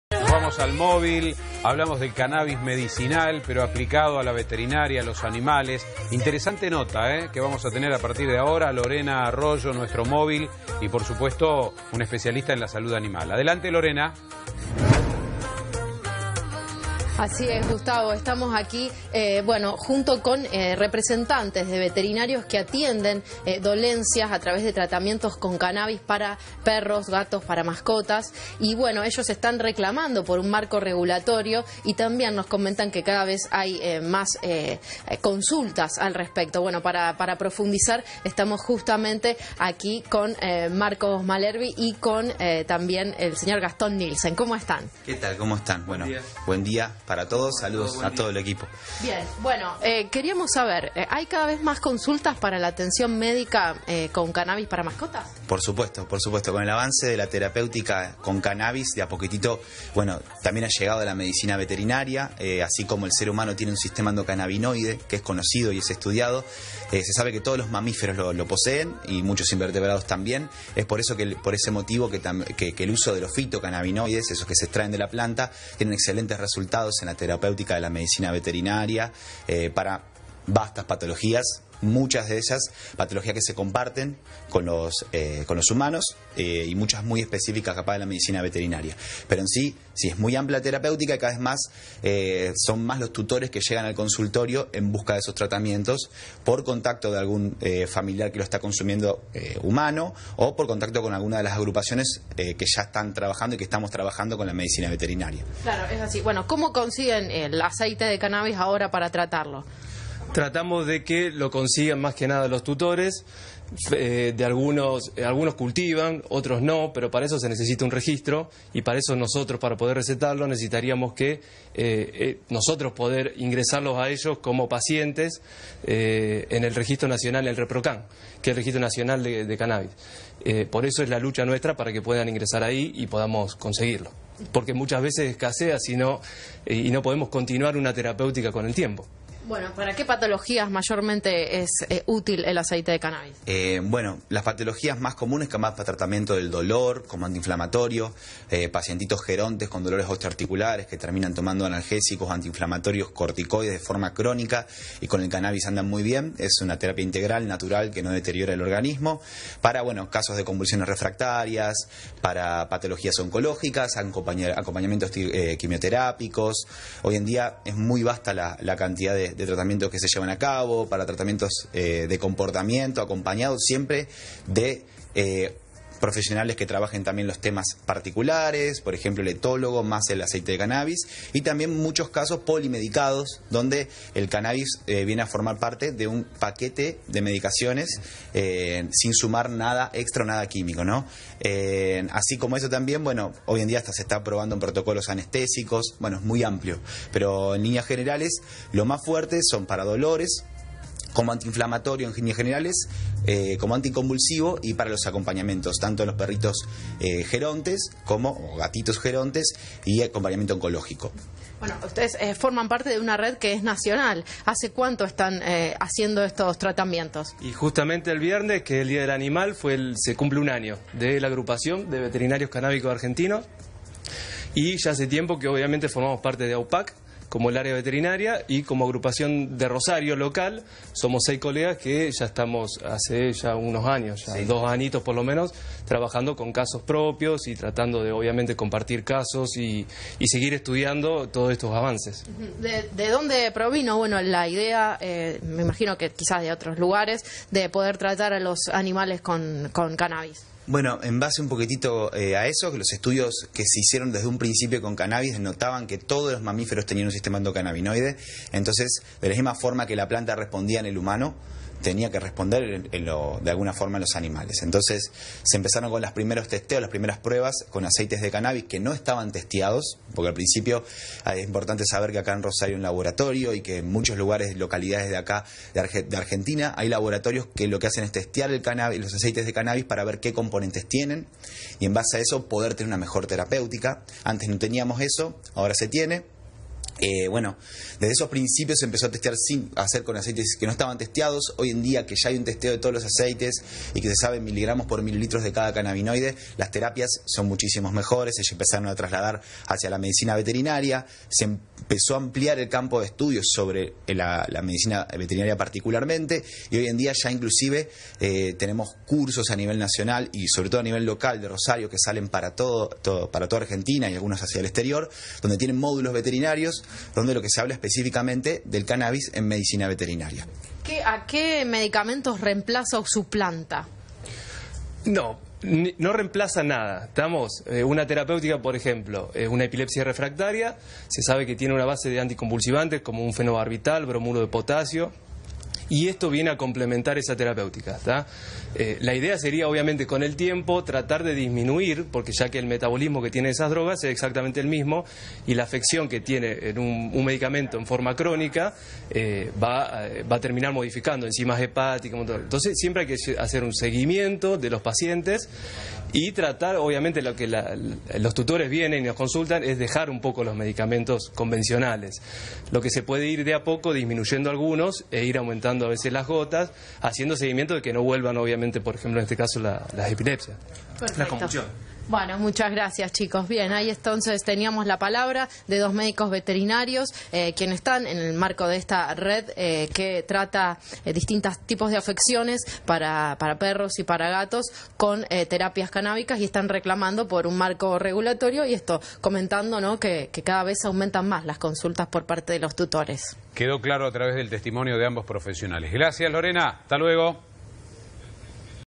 Cada vez son más frecuentes los tratamientos con cannabis para mascotas. Telefe Rosario.
Marcos Malerbi y Gastón Nielsen, médicos veterniarios rosarinos de Vedtcra explican sobre el uso y aplicación del cannabis en animales y reclaman un marco regulatorio.